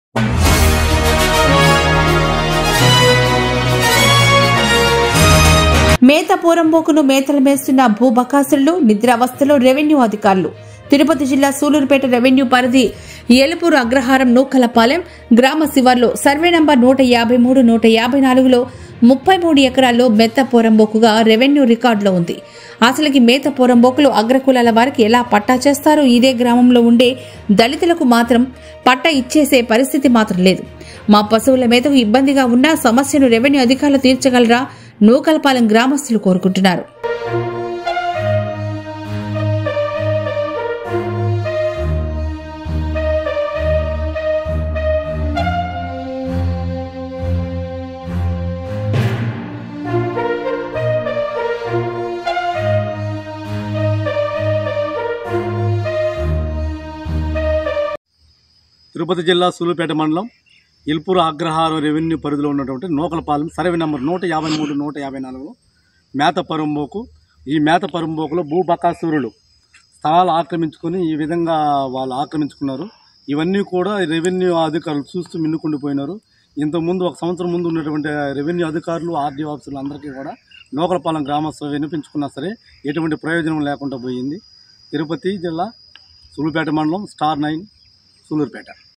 STAR9 LIVE మేతపూరంబోకును మేతల మేస్తూన్న భూబకాసురులు, నిద్రావస్థలో రెవెన్యూ అధికారులు
మేతపూరంబోకును మేతల మేస్తూన్న  భూబకాసురులు,  నిద్రావస్థలో  రెవెన్యూ అధికారులు